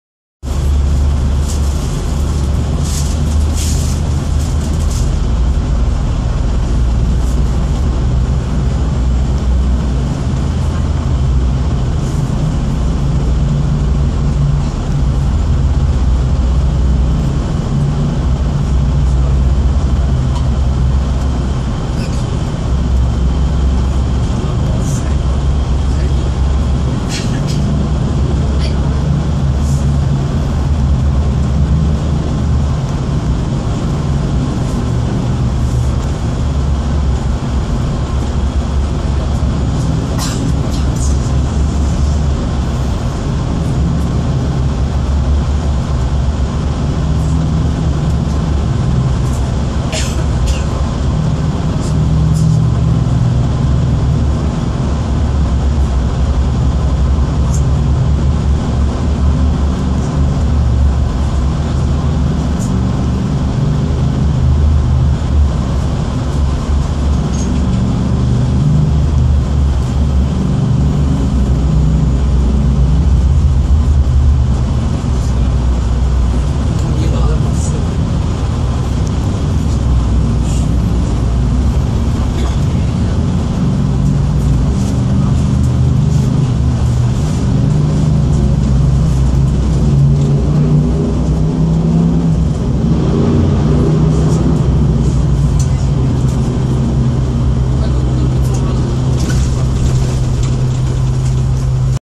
THE FIRST TUNNEL BUILT UNDERWATER in HONGKONG
The Cross-Harbour Tunnel is the first tunnel in Hong Kong built underwater. It consists of two steel road tunnels each with two lanes constructed using the single shell immersed tube method. It is the earliest of three vehicular harbour crossings in Hong Kong, opened for traffic on 2 August 1972. Wikipedia